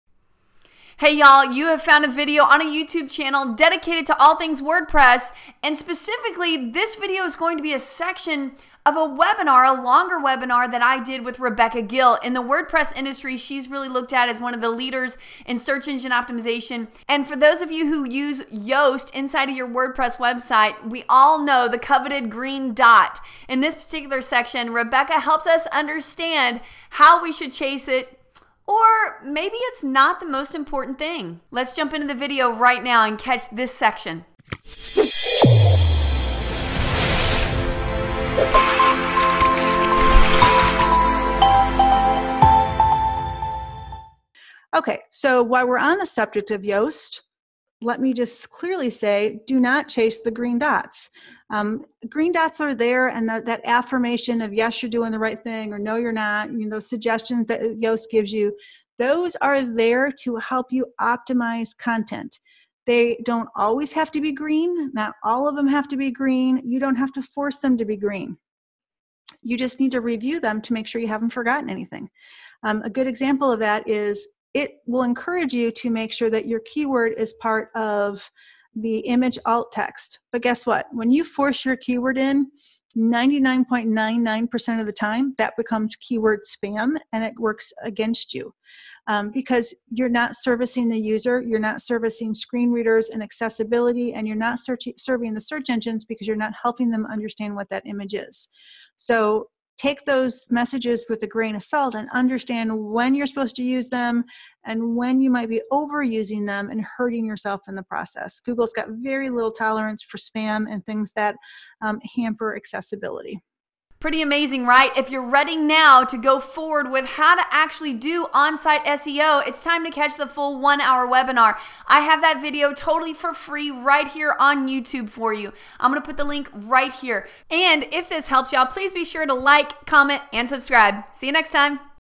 💚💚 Yoast - Getting to Green Lights in Your WordPress Website
This video is a shorter clip from a longer webinar hosted by Kori Ashton and special guest Rebecca Gill.